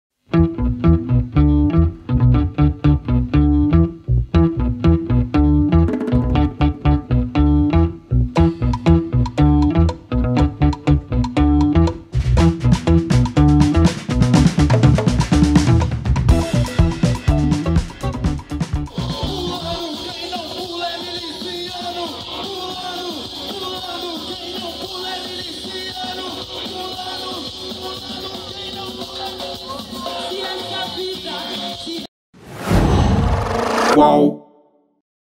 'Quem não pula é miliciano': banda transforma Rock in Rio Lisboa em protesto
No segundo final de semana do Rock in Rio Lisboa a banda brasileira ‘Francisco El Hombre’ fez um show com mensagens políticas e de críticas ao presidente Jair Bolsonaro (PL).